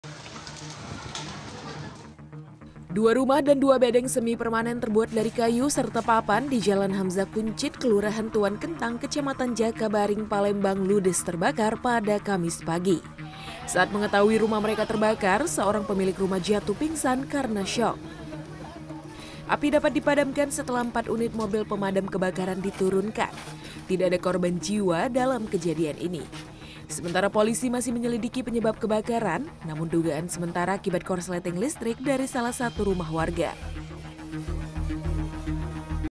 2 Rumah dan 2 Bedeng Terbakar, Pemilik Histeris dan Pingsan
Dua rumah dan dua bedeng di Palembang, Sumatera Selatan hangus terbakar. Akibat kejadian ini, pemilik rumah histeris hingga jatuh pingsan.